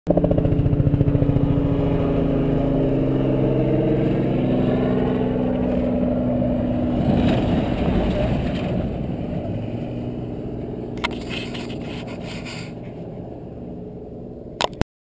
2x chase crew WRONG WAY ON THE COURSE RM65 Baja 500 2011
This idiot thought it was ok to go the wrong way on the course. 2x was DQ'd because of this...good